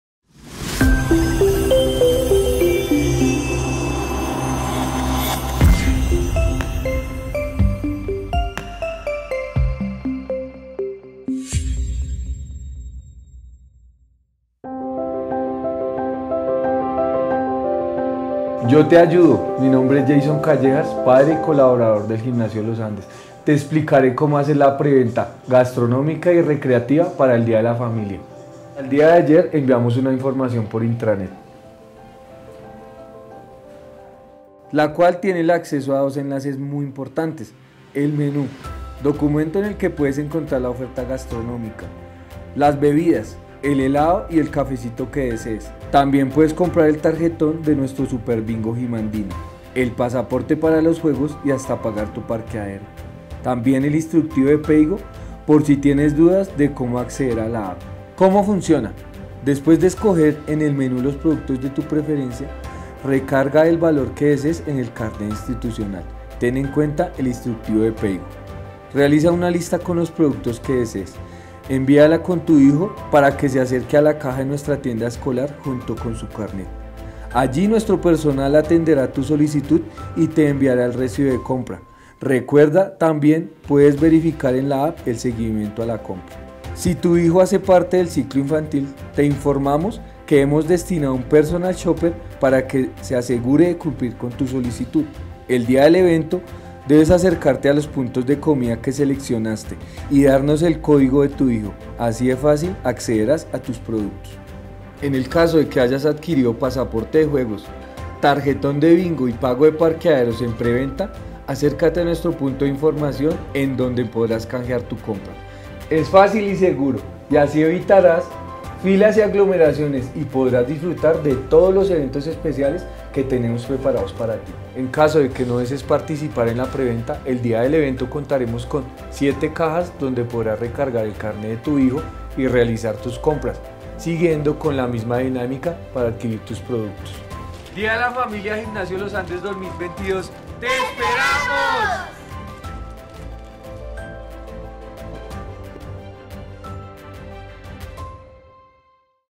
PREVENTA DÍA DE LA FAMILIA 2022
¿Tienes dudas sobre el día de la familia?
A continuación, te explicamos cómo hacer la preventa gastronómica y recreativa para el día de la familia.